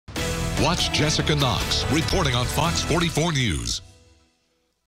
WGMB Station ID 2020
This is WGMB's station ID from 2020
WGMB is located in Baton Rouge, LA